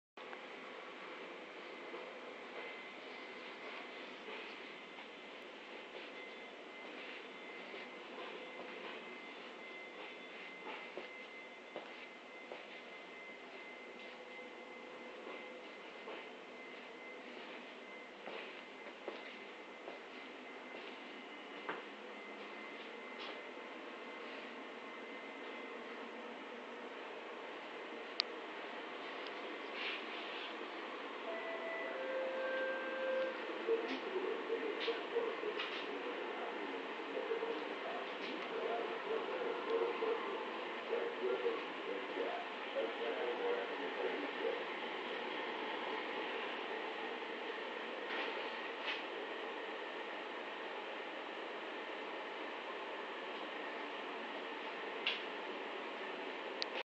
Formacion china con luz de la Linea San Martin (SOFSE)
Aca podran ver la formacion china que esta prestando servicio en la Linea San Martin con las luces prendidas a cargo de la locomotora SDD7 B966 en Retiro